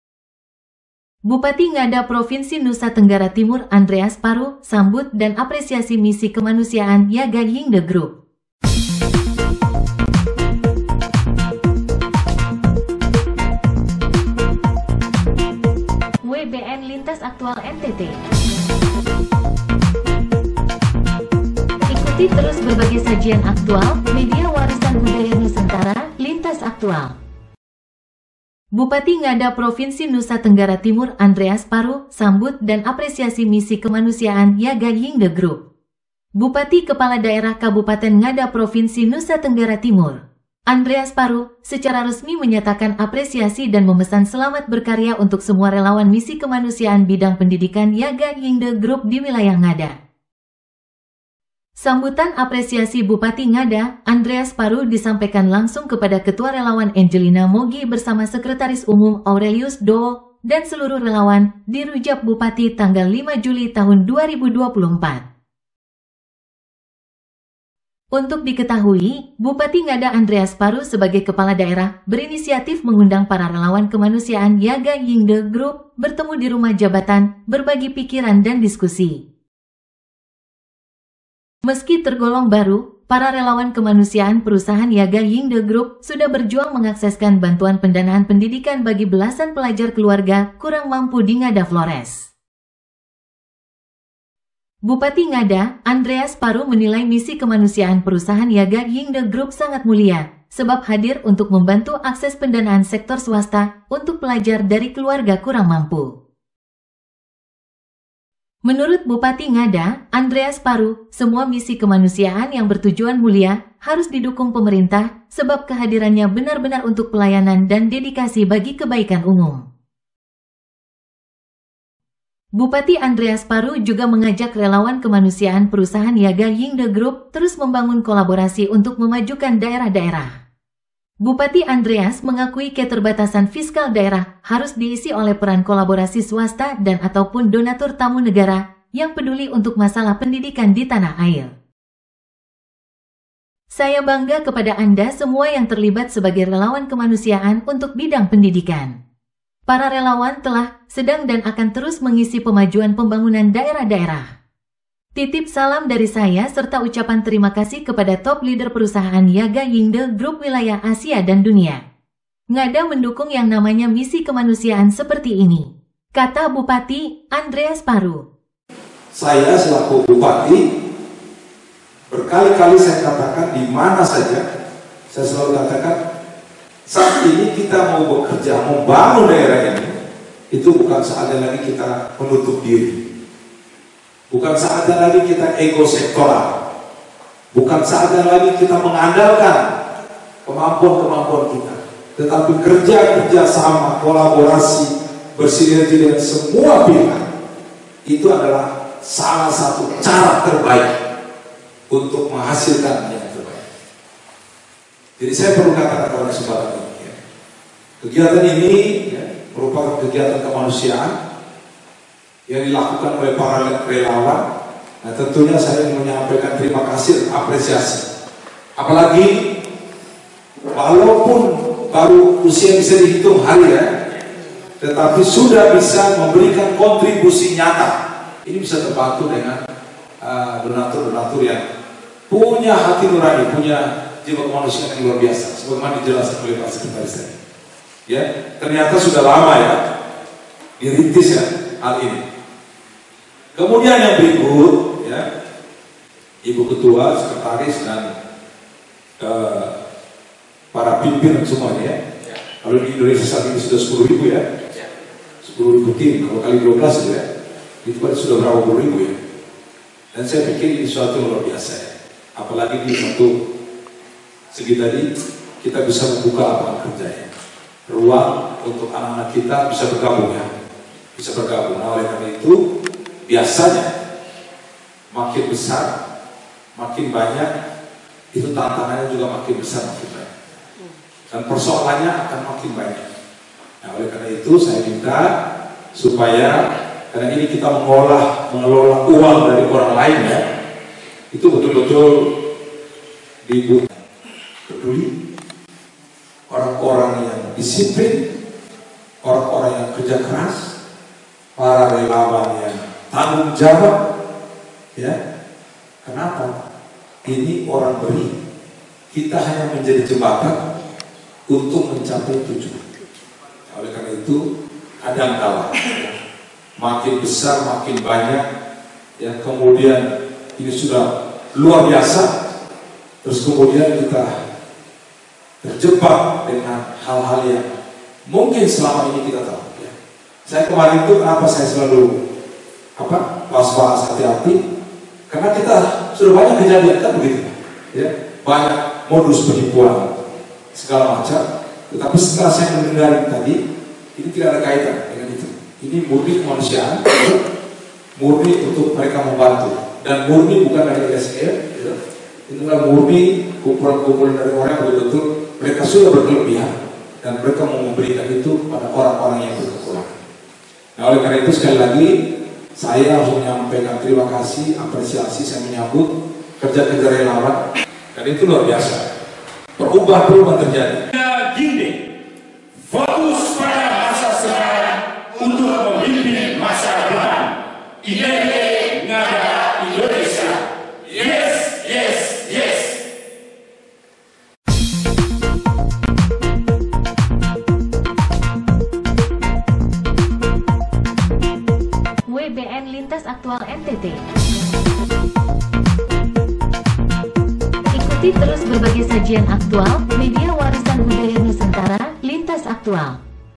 BUPATI NGADA NTT APRESIASI YAGA YINGDE MISI KEMANUSIAAN
Bupati Kepala Daerah Kabupaten Ngada Provinsi Nusa Tenggara Timur, Andreas Paru secara resmi menyatakan apresiasi dan memesan selamat berkarya untuk semua Relawan Misi Kemanusiaan Bidang Pendidikan Yaga Yingde Grup di wilayah Ngada.

Sambutan apresiasi Bupati Ngada Andreas Paru disampaikan langsung kepada Ketua Relawan Angelina Mogi bersama Sekretaris Umum Aurelius Do’o dan seluruh relawan, bertempat di Rujab Bupati Ngada Flores, Jumat 5 Juli 2024.

Untuk diketahui, Bupati Ngada Andreas Paru sebagai Kepala Daerah berinisiatif mengundang para Relawan Kemanusiaan Yaga Yingde Grup bertemu di rumah jabatan 'berbagi pikiran dan diskusi.

Meski tergolong baru, para relawan kemanusiaan Perusahan Yaga Yingde Grup sudah berjuang mengakseskan bantuan pendanaan pendidikan bagi belasan pelajar keluarga kurang mampu di Ngada Flores.

Bupati Ngada Andreas Paru menilai misi kemanusiaan Perusahan Yaga Yingde Grup sangat mulia, sebab hadir untuk membantu akses pendanaan sektor swasta bagi pelajar atau anak sekolah dari keluarga kurang mampu.

Menurut Bupati Ngada, Andreas Paru, semua misi kemanusiaan yang bertujuan mulia, harus didukung oleh pemerintah, sebab kehadirannya benar-benar untuk pelayanan dan dedikasi bagi kebaikan umum.

Bupati Andreas Paru juga mengajak Relawan Kemanusiaan Perusahan Yaga Yingde Grup untuk terus membangun kolaborasi demi memajukan daerah-daerah.

Bupati Andreas mengakui keterbatasan fiskal daerah harus diisi oleh peran kolaborasi swasta dan ataupun donatur tamu negara yang peduli untuk masalah pendidikan di tanah air.

"Saya bangga kepada anda semua yang terlibat sebagai relawan kemanusiaan untuk bidang pendidikan. Para relawan telah 'sedang dan akan terus mengisi pemajuan pembangunan daerah-daerah. Titip salam dari  saya serta ucapan terimakasih kepada Top Leader Perusahaan Yaga Yingde Grup wilayah Asia dan dunia. Ngada mendukung yang namanya misi kemanusiaan seperti ini", kata Bupati 'Andreas Paru, Jumat (05/07/2024).

WBN News